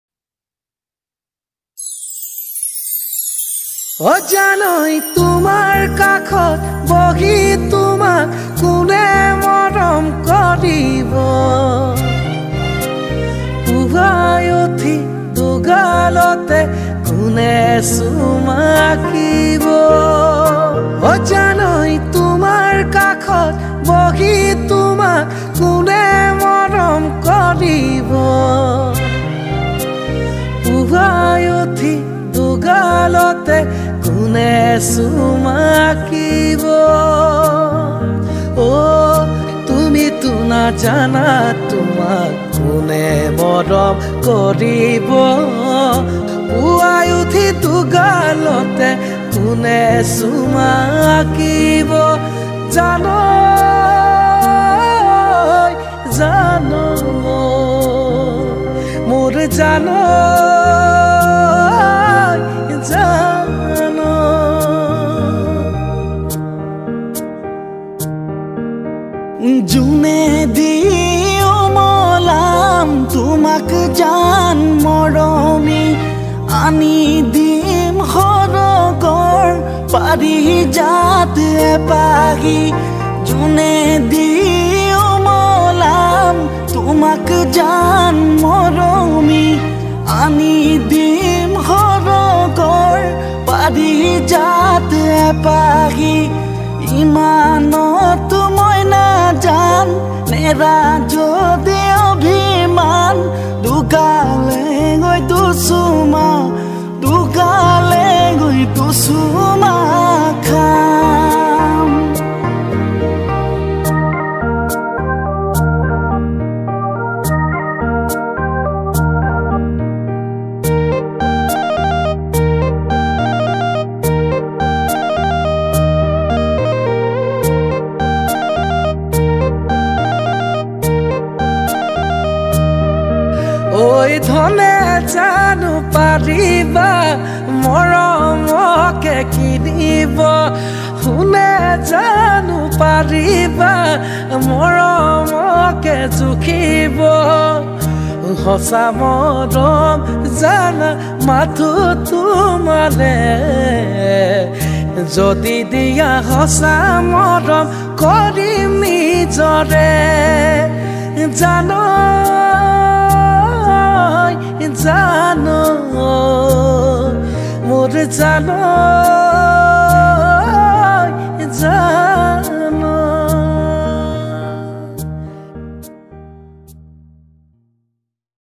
cover song by akash nibir biswajit khatowal
Music/Bhaskar Nelom
singer/ Akash nibir/Biswajit khatowal
mixed mastering/Bhaskar Nelom
studio/strings tengakhat
spsl thnx/Joonkar mousumi,samiran,tridip saikia,pinku kashyap,all assam student Union, Naharkatia aasu.news live,pratidin time,